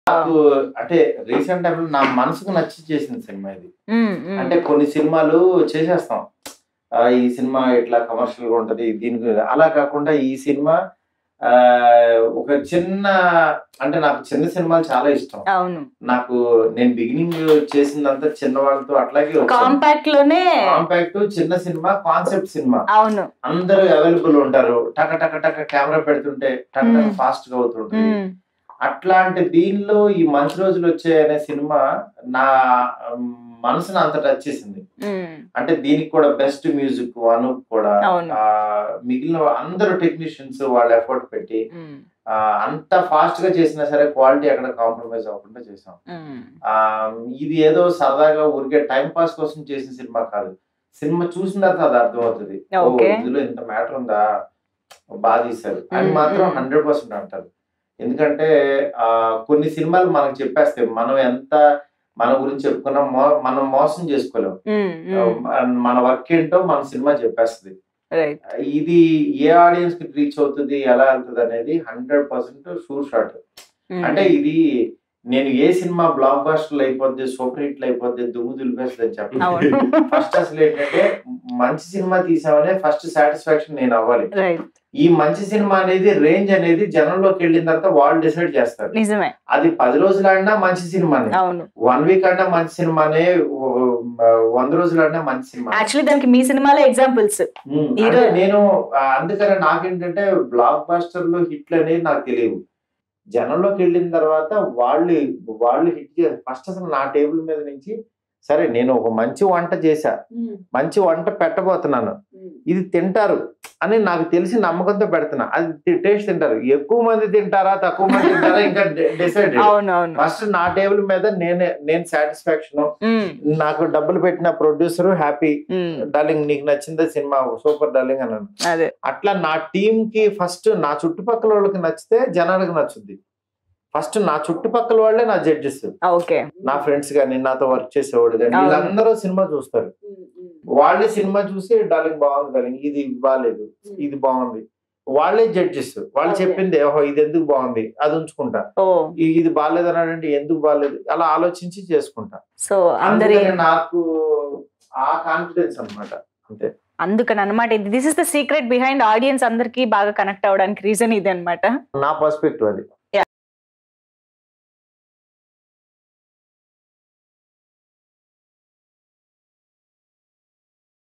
Director Maruthi Saying About Manchi Rojulu Vachai Movie Tollywood Update
WATCH:-Director Maruthi Saying About Manchi Rojulu Vachai Movie Tollywood Update